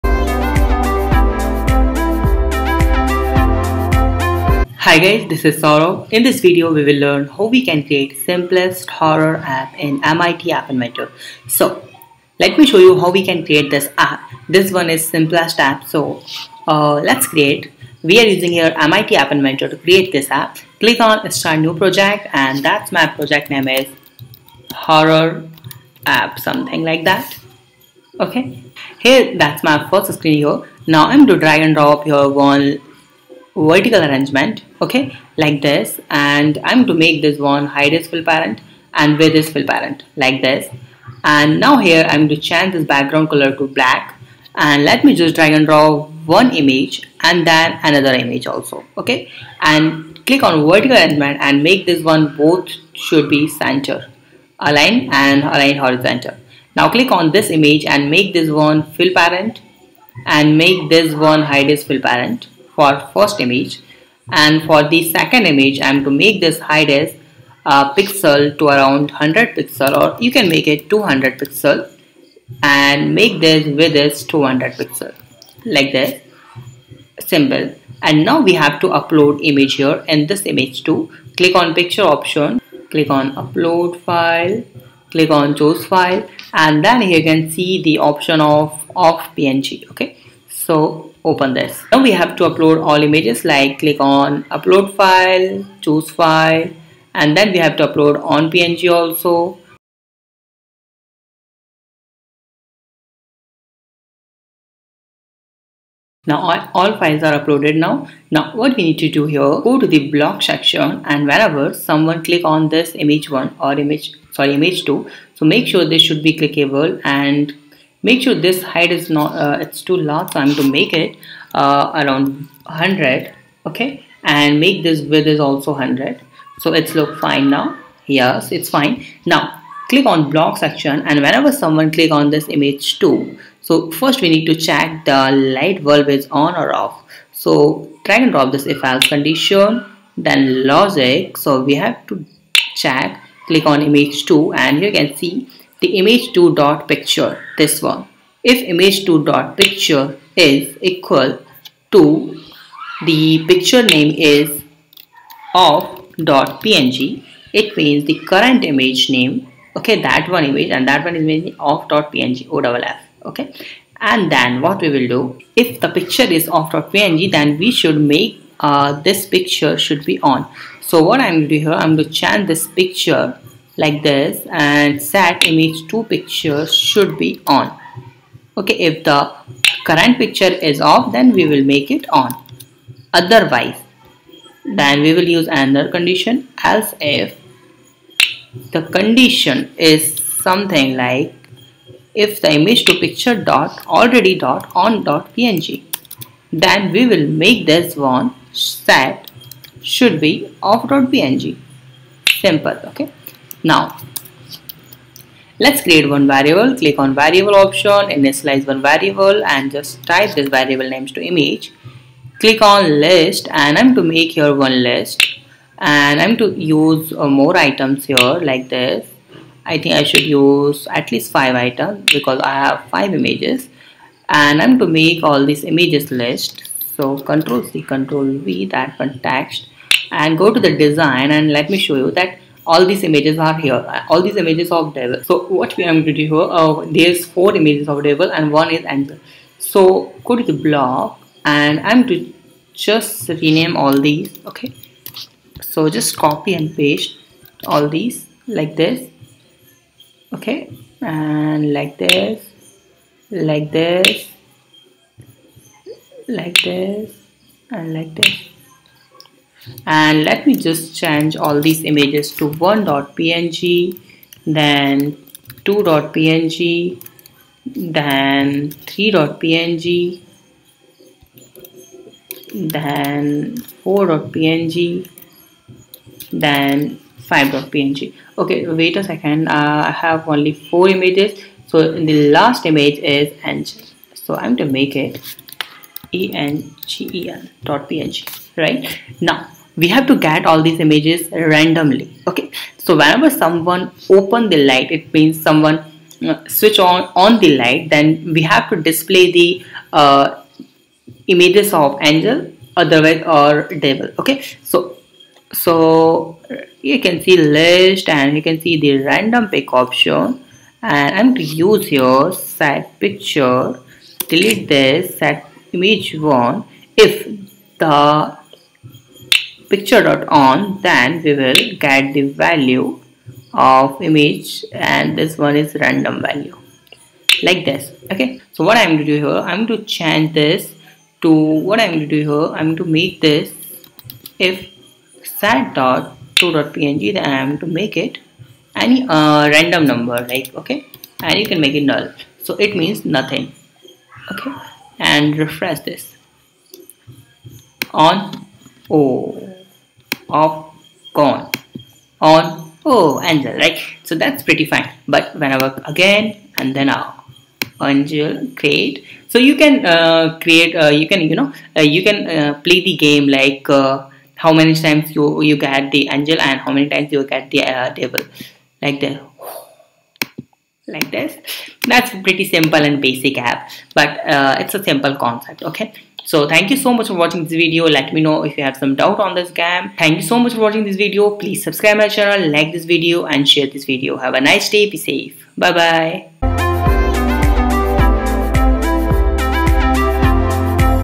How to create Horror Game in MIT App Inventor 2
How to make Horror Game in MIT App Inventor 2

mit app inventor extensions
mit app inventor gps tracking
mit app inventor drawing app
mit companion ai2